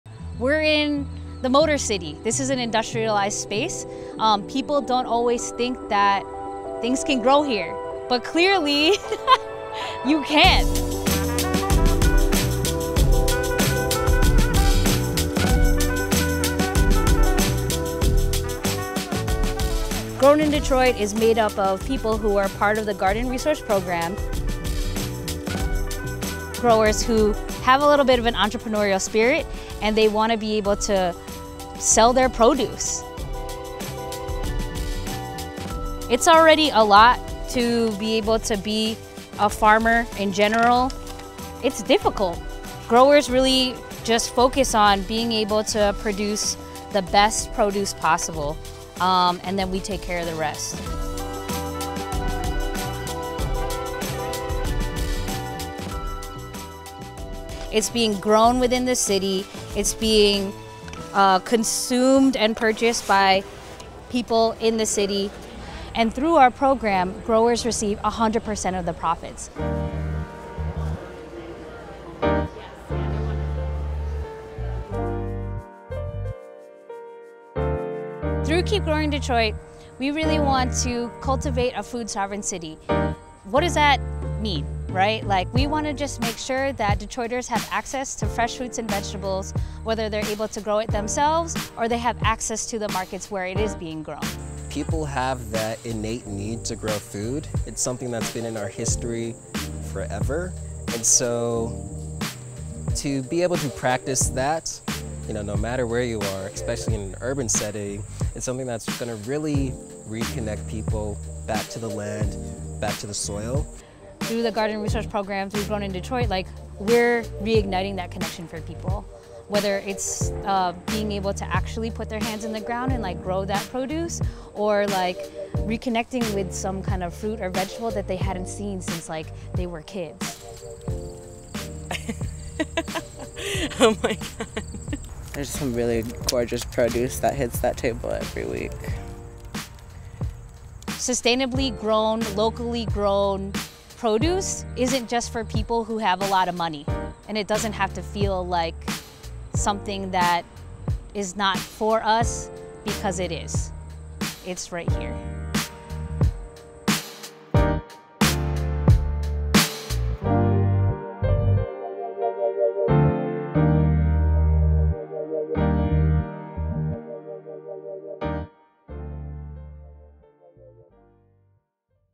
Intro to Keep Growing Detroit
Learn more about Keep Growing Detroit and their Grown in Detroit project with this intro video!